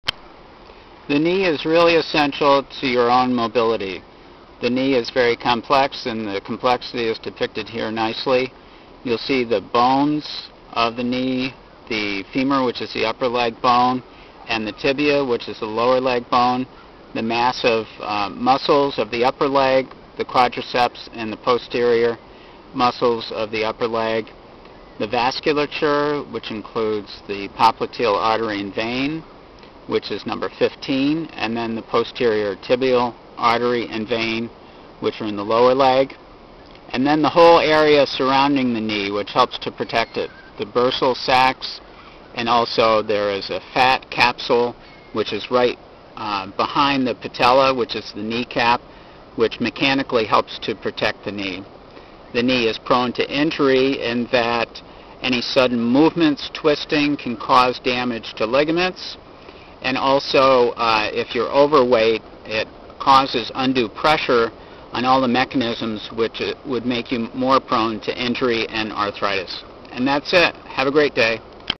"Anatomy and Physiology", The Joints of the Body, The Knee
The Knee plays a fundamental role in the mobility of human beings. The Knee Joint integrates the complexity of interplay of the primary bones of the upper leg, the femur, and the lower leg, the tibia. In addition, the Knee functions in concert with the massive muscle tissues of the upper and lower leg. The Knee Joint itself acts to absorb stresses via bursal tissues, and a fat sac that is posterior to the patella (the knee cap). A complex array of tendons act to stablize the knee in all levels of activity, from normal walking, to high level competitive Sports. The Knee is prone to overuse injuries, torsional injuries, as well as, injuries due to extreme stressors, commonly caused by being overweight.